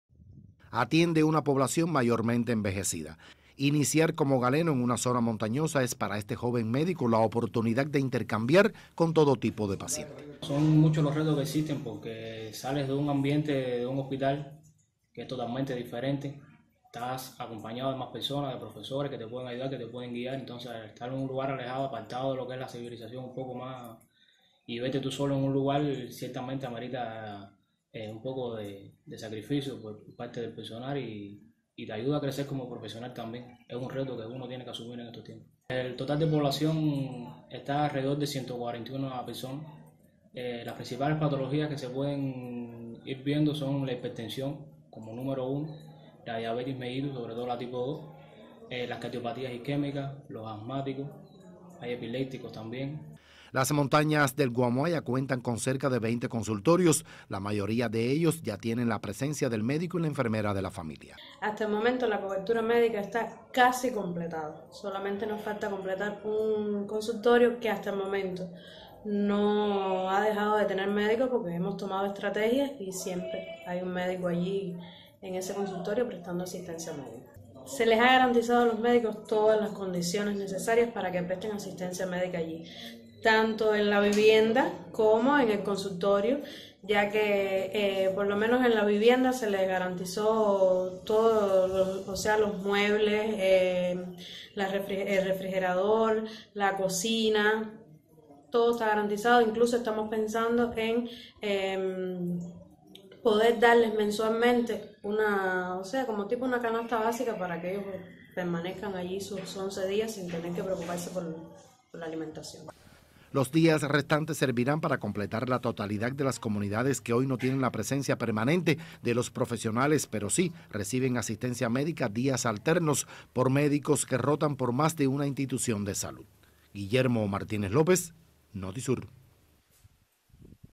Asegurada la asistencia médica en montañas de Cienfuegos
Con más de 15 consultorios del Médico y la Enfermera de la Familia, se garantiza la asistencia sanitaria de la población del Consejo popular  de San Blas, en Cumanayagua. En Hoyo de Padilla, por ejemplo, un joven profesional inicia sus labores en las montañas de Cienfuegos.